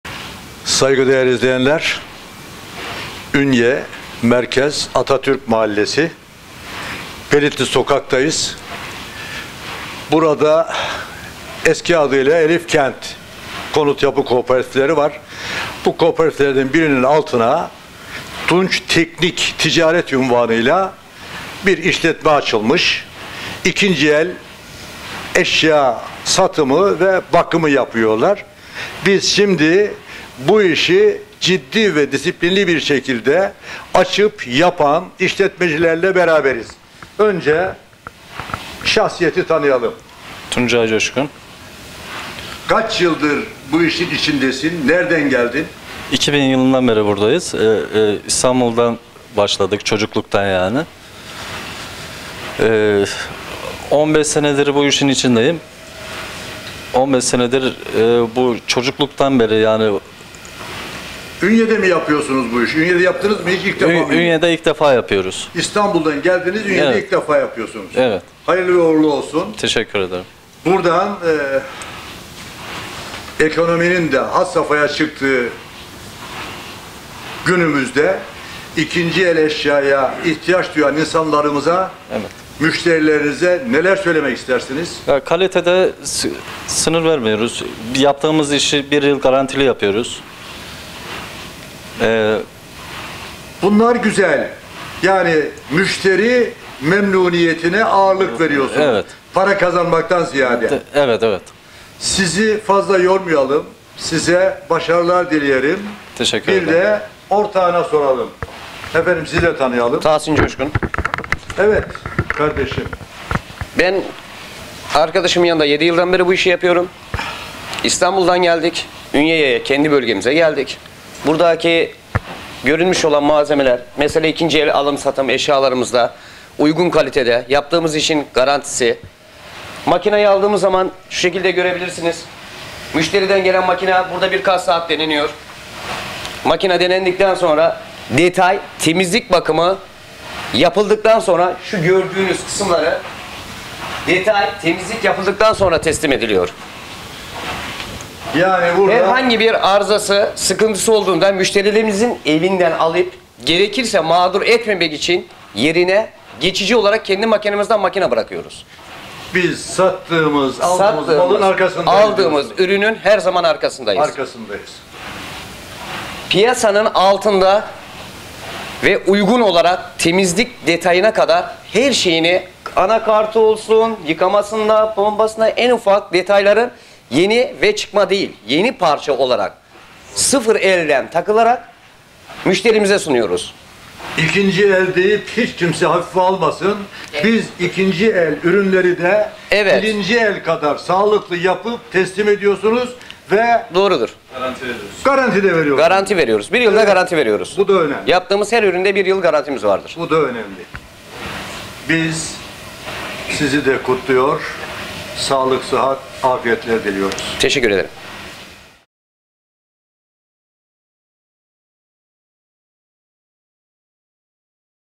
2. El Ürünlere Sıfır Gibi Garanti
Coşkun” Tunç Teknik 2’ci El Beyaz Eşya Alış,Satış Servisi Hizmete Açıldı”

 Ünye merkez Atatürk Mahallesi Pelitlik Soka No2/A Ünye adresinde “ Tunç Teknik ” Ticaret unvanı ile beyaz eşya alış satış ve Poyraz Temizlik işletmesi bugün Ünyelilerin ve Ünye’de yaşayan tüm ihtiyaç sahibi insanların hizmetine açıldı.